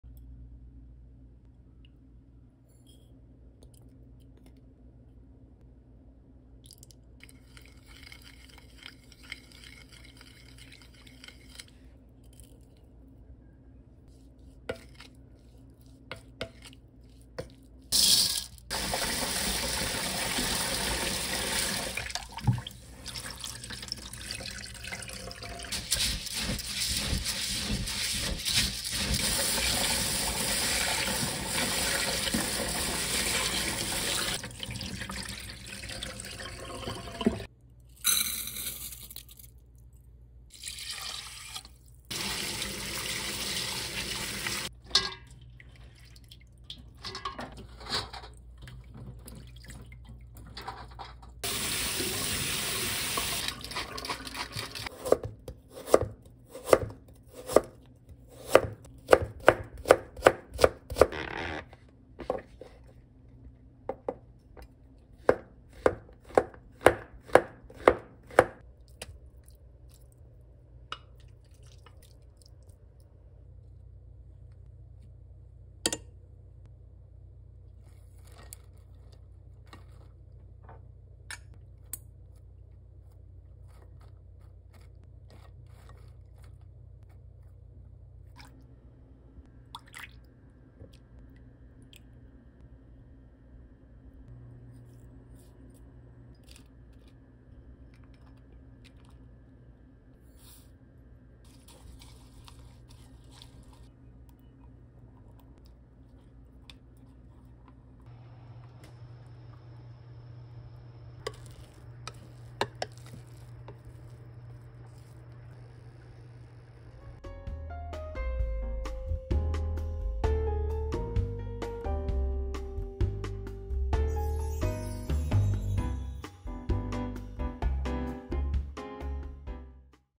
실패 없는 콩나물밥ㅣ콩나물밥을 압력밥솥(전기밥솥)으로 간단하게 만드는 법! 맛있는 콩나물밥 양념장 레시피까지!
오늘은 콩나물밥 압력밥솥 (전기밥솥) 레시피를 알려드릴게요!

콩나물 200g
쌀(백미) 2컵
소고기 다짐육 130g
당근 20g
표고버섯 1~2개

*콩나물은 취향따라 꼬리는 제거하고 지저분한 콩나물 대가리는 떼어내주세요

*소고기양념장
설탕 1/2큰술
다진마늘 1/2큰술
참기름 1큰술
후추 약간